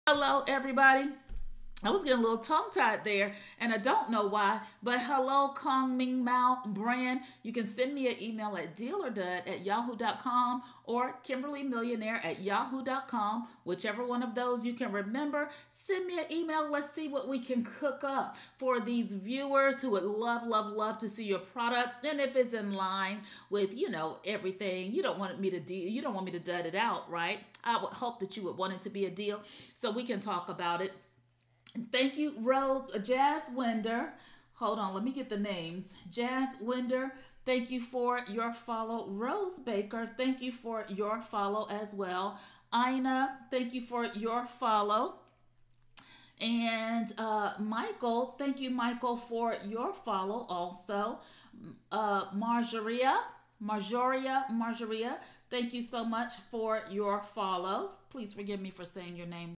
Deal or Dud with Kimberly Millionaire on Amazon Live - 12/5/2022
Deals:
6:10 - - MECHEER Over The Sink Dish Drying Rack
48:45 - - Whiskey Decanter Globe Set
1:15:27 - - Cheese Board Set
1:30:50 - - BodyRestore Shower Steamers
1:37:53 - - SAMSUNG Galaxy Watch
1:42:06 - - Flameless Led Candles
1:45:36 - - eufy by Anker
1:50:54 - - Timekettle M2 Language Translator Earbuds
1:55:50 - - USB Microphone with Tripod

Please note: Prices accurate at the time of recording.